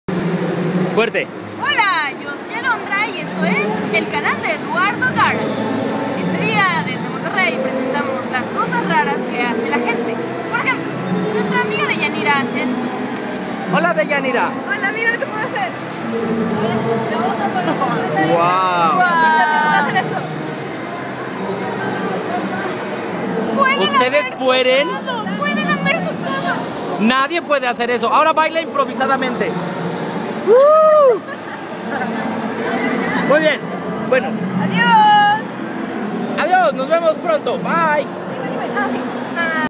Alondra Hidalgo en iEduardoGaara
Desde Monterrey con Deyanira, una chica que hace cosas muuuuy raras.